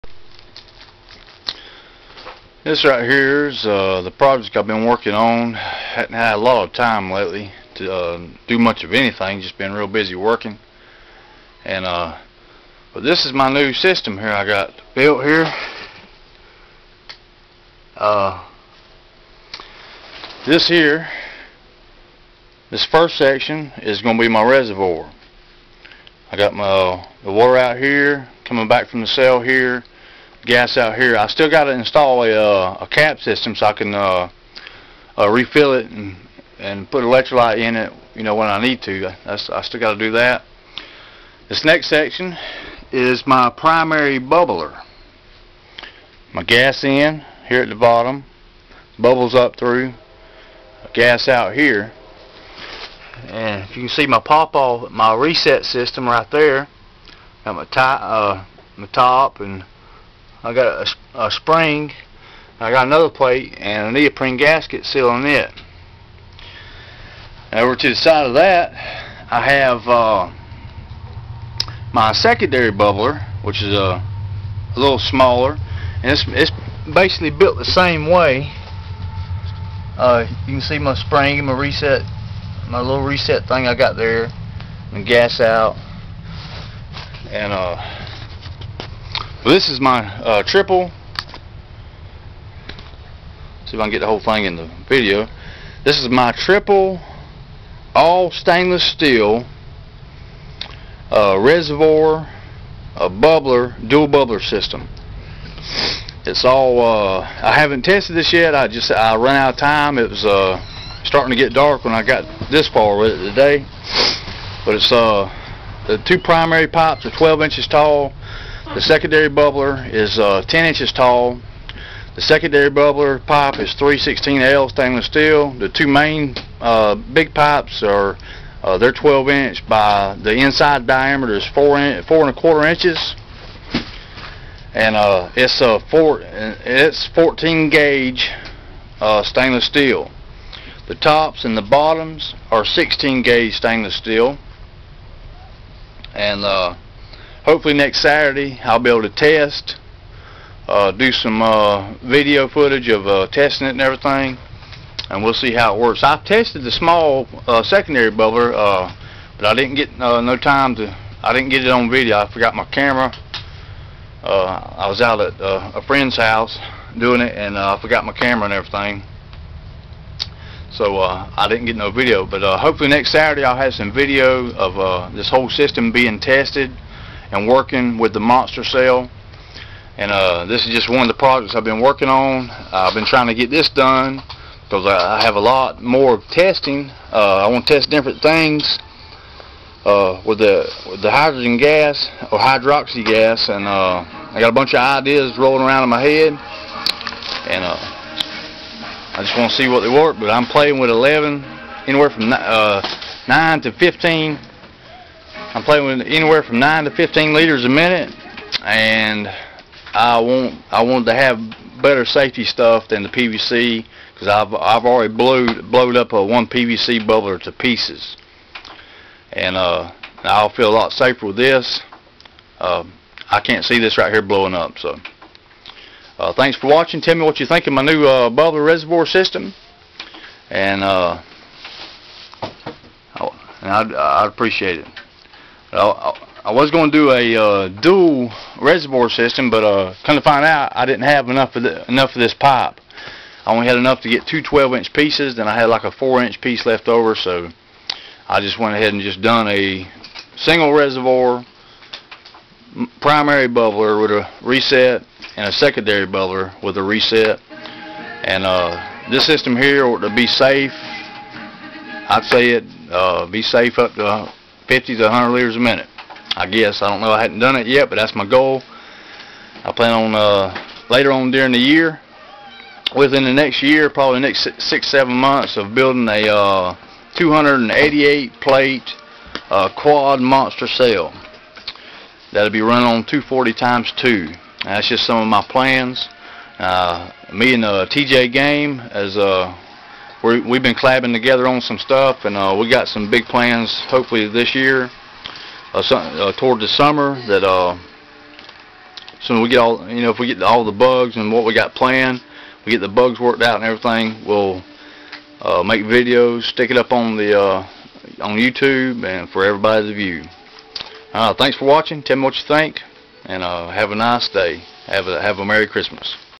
The monster cell Triple Bubbler reservoir system
This my bubbler/reservoir system ,I have been working on for the last 2 weekends..Time has been tight ,but I been sqeazing in as much as i can.
This setup has a
12" tall 4 1/4" wide reservoir   14  gauge 304 stainless pipe
12" tall 4 1/4" primary bubbler  14  gauge 304 stainless pipe
10" tall 2 1/2" secondary bubbler 18 gauge 316 stainless pipe..

The unit has not been tested yet , but will have all test results next weekend..